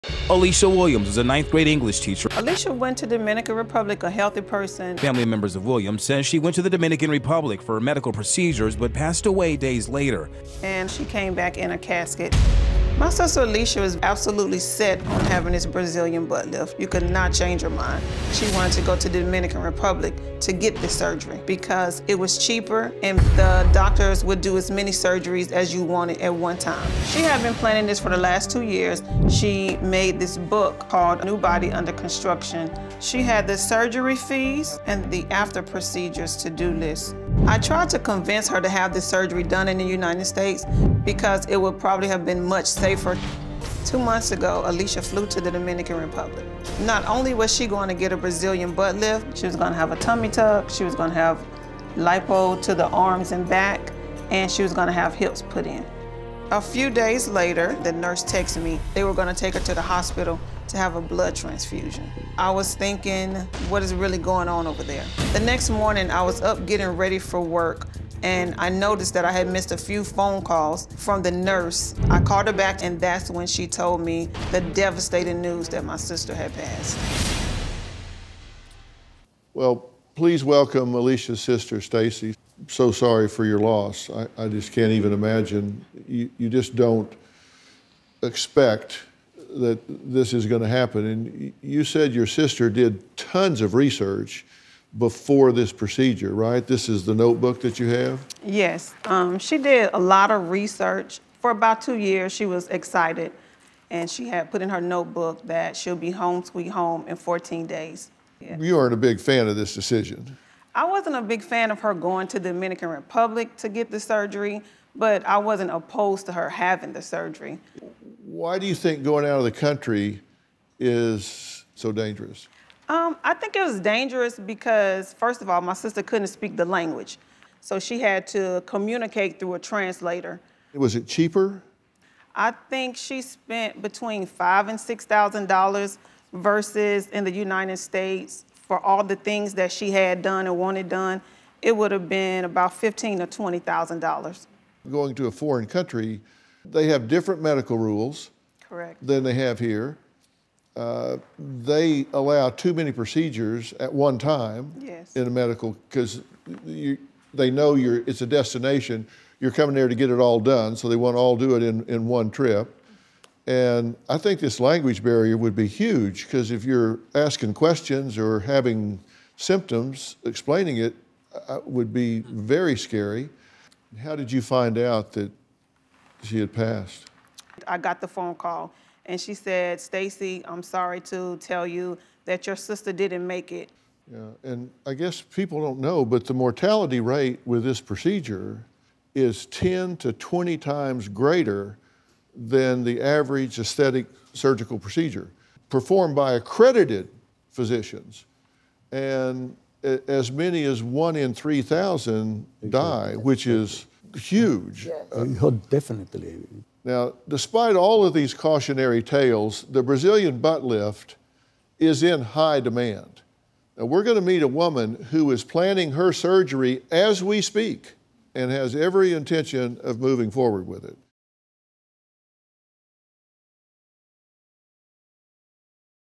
Teacher Dies Following Plastic Surgery In The Dominican Republic
A woman whose 45-year-old sister died after having plastic surgery overseas says she tried to convince her sibling not to go.